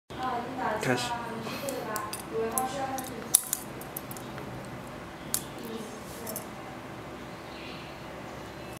How to fit Starline & Vistapro Wiper Blades
How to fit Starline & Vistapro Side Pin Wiper blades from EuroCarParts